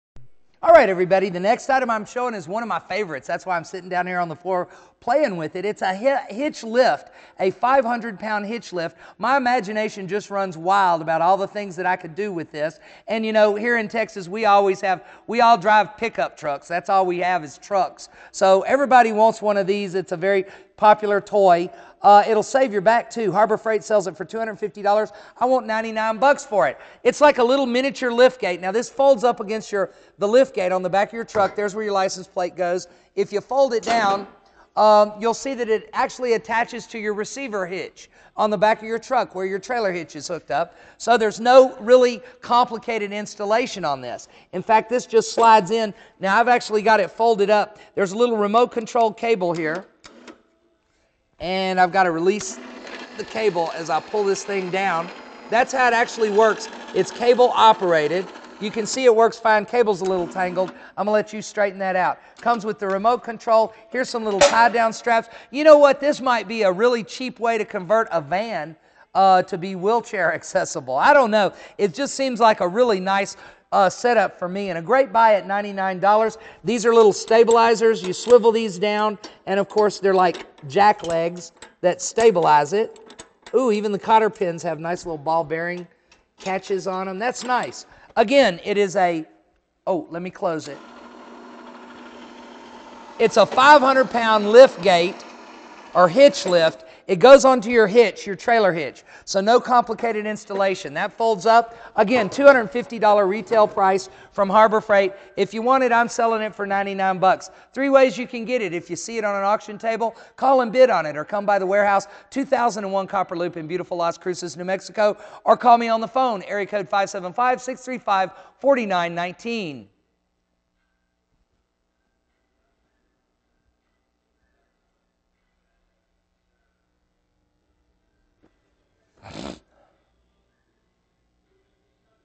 #3252 500lb Hitch Lift $99
Item for sale in the KbidTV warehouse at 2001 Copper Loop Las Cruces, NMFrom Harbor Freight Reg $249